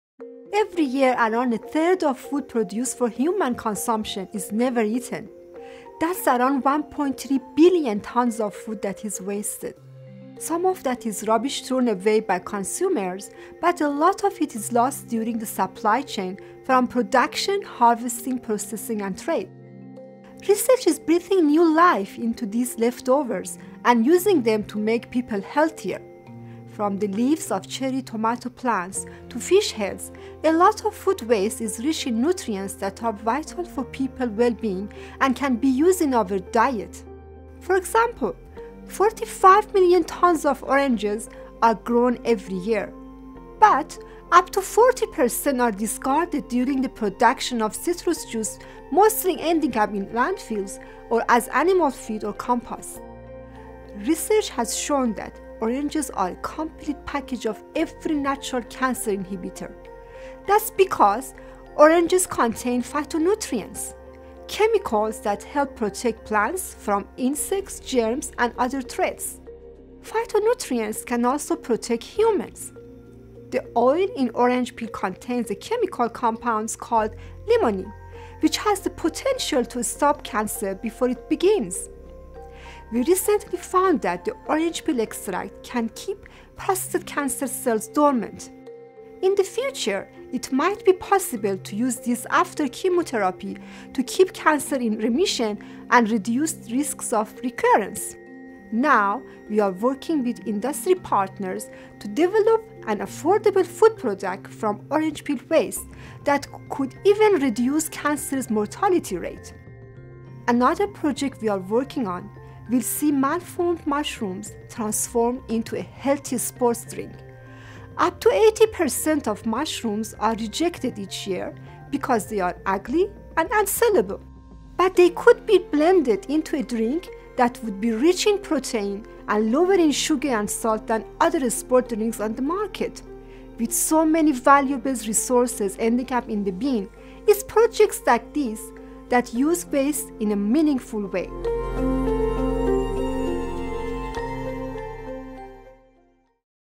Explainer: Transforming food waste
1.3 billion tonnes of food is wasted every year. Meet Professor Fariba Dehghani, one of the scientists turning these scraps into life-saving medicine. Produced in association with the Sydney Morning Herald.